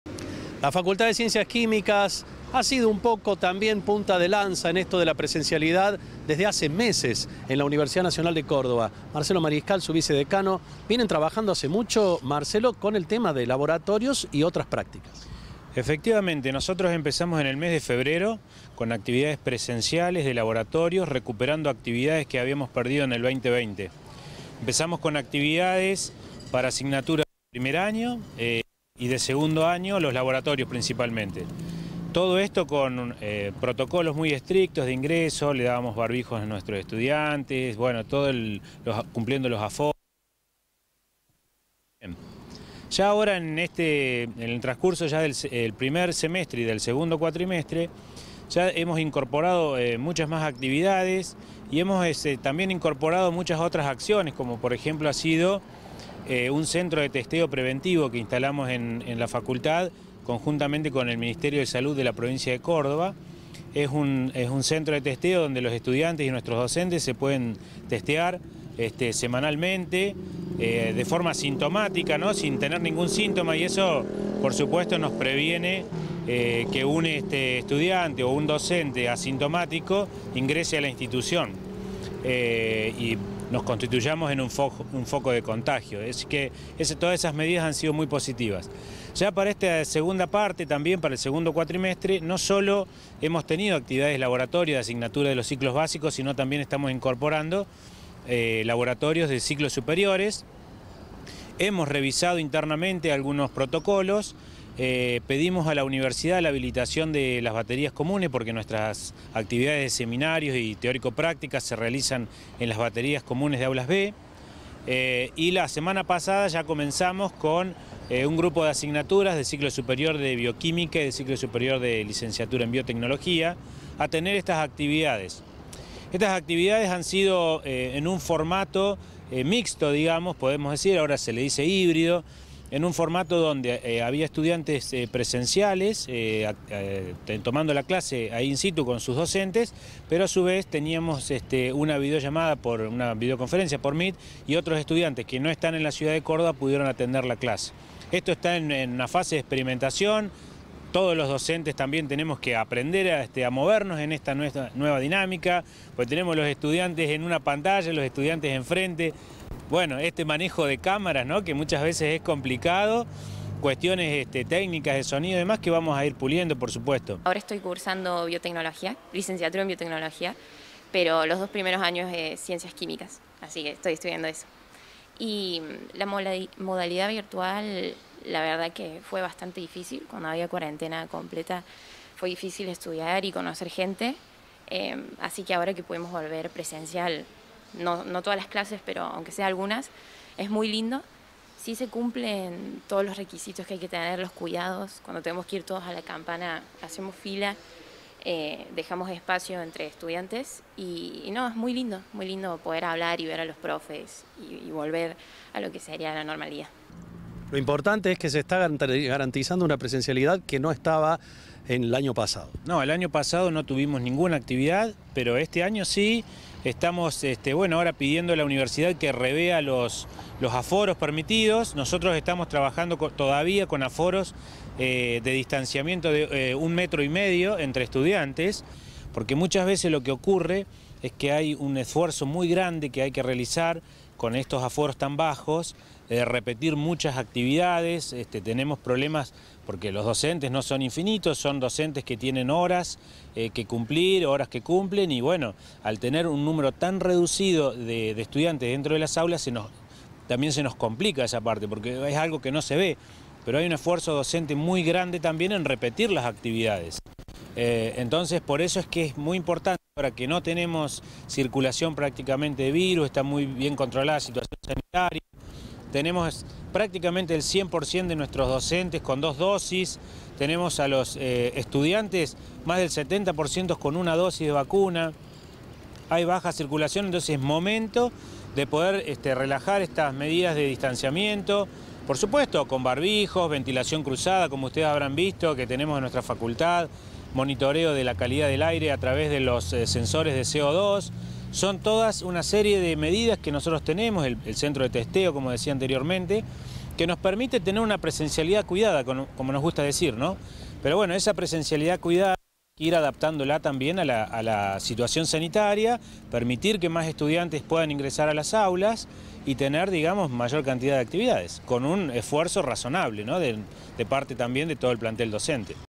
Estudiantes de la UNC regresan a sus facultades
Según pudimos relevar, es importante la cantidad de estudiantes que ya volvieron a las aulas, mientras que quienes no pueden hacerlo, mantendrán el sistema virtual o mixto.